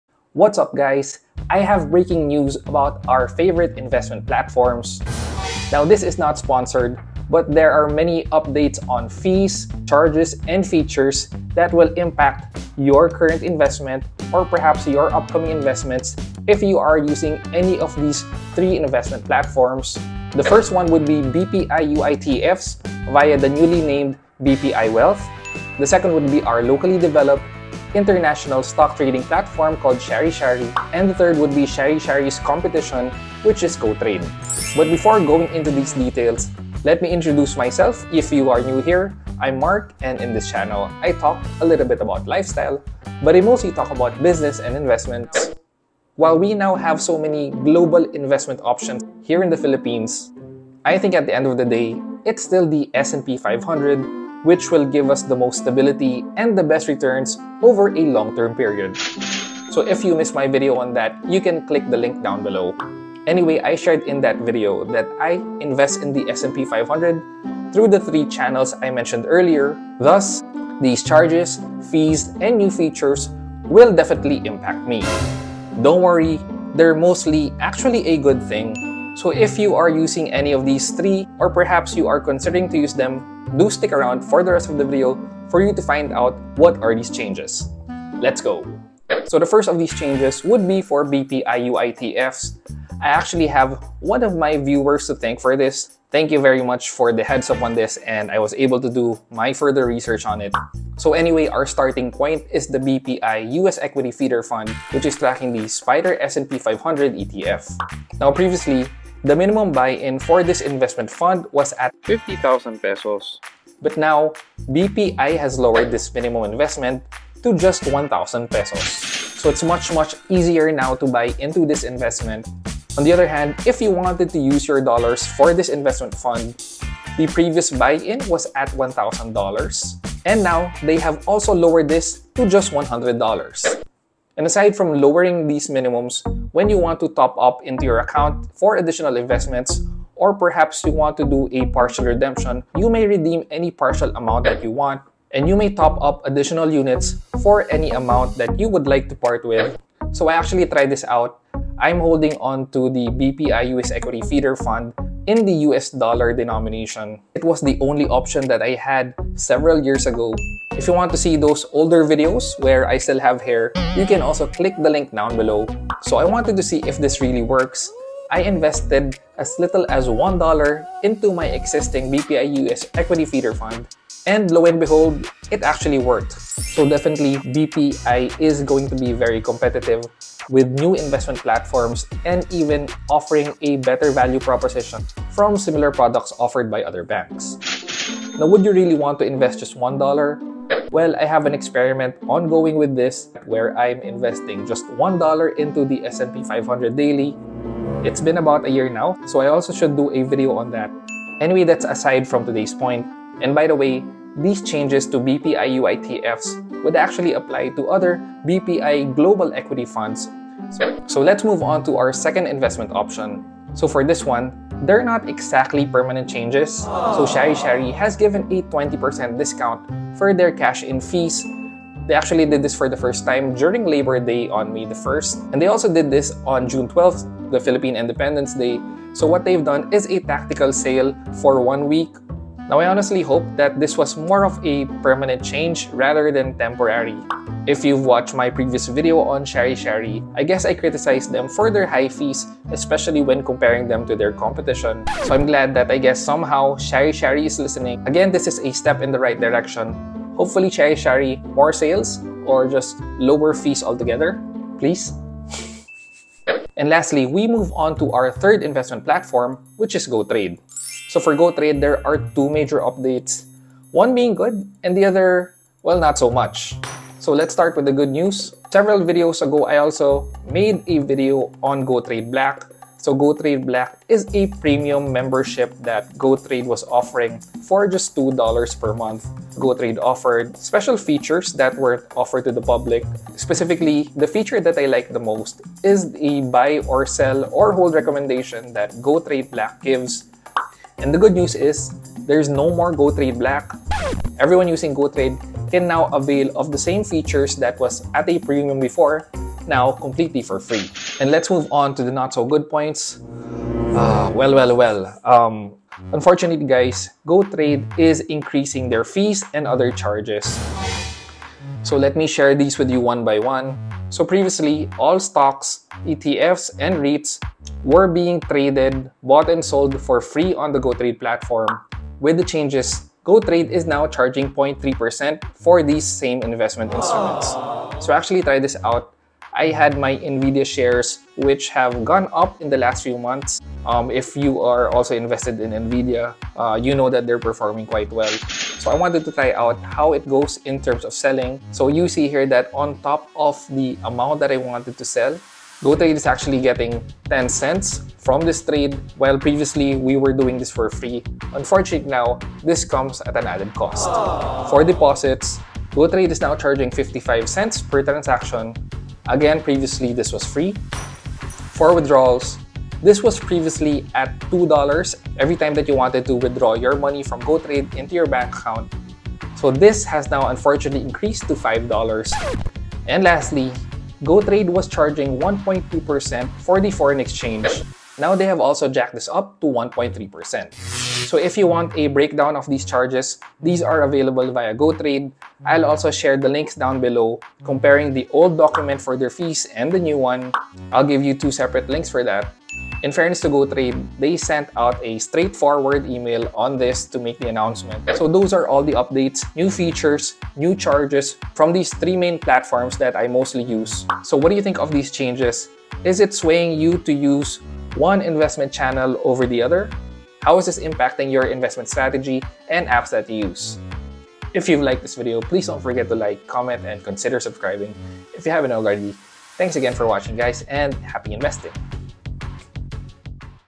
BPI WEALTH, SHARI-SHARI, GOTRADE: Updates on Charges, Fees, & Features 2023!
It's no secret that I prefer investing in the US and Global Markets as shared in many of my videos throughout the years! I use a number of platforms including GCash GInvest, BPI UITFs, ALFM Mutual Funds, Seedbox Philippines, ATRAM, Gotrade, and Shari-Shari. There are however major updates this 2023 on the fees, charges, and features of the three main platforms that i use/ BPI Wealth, Gotrade, and Shari-Shari. While seemingly minor, the updates will affect my current investment positions and investment strategy moving forward. Moreso as i invest in the S&P 500 routinely, the changes in these investment platforms will surely impact me. I'm sure it will infect your current and upcoming investments too so watch on to see how you can handle your investments moving forward.

WHY I CONTINUE TO INVEST IN THE US MARKET EVEN IN TIMES OF WAR | US STOCKS OVER PHILIPPINE STOCKS

Sub-count: 5,505